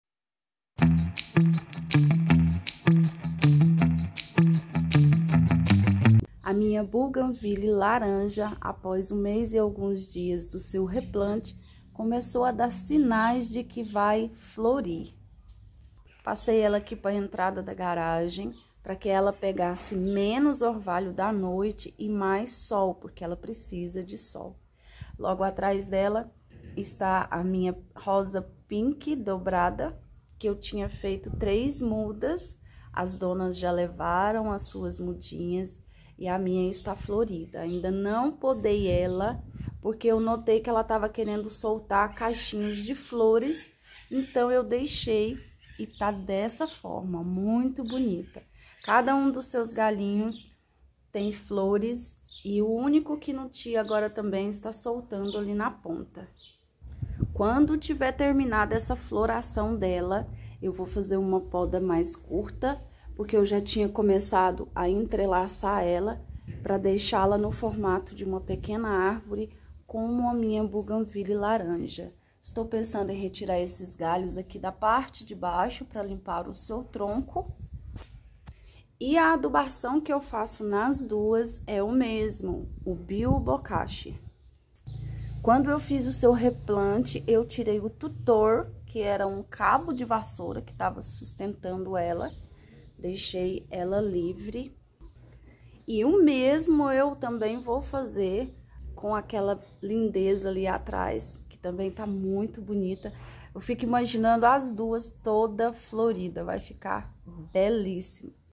Bougainville laranja e rosa pink dobrada
Depois de fazer o replante das duas após quase 2 mêses, elas estão florindo novamente, mudei elas de lugar para evitar o orvalho da noite que junto com frio  e o sol durante o dia faz requeimar suas folhas, pelo menos da variegata laranja, a rosa pink tem mais resistência pelo que observei.
Ambas estão lindas, em breve terei uma roxa para fazer companhia a elas.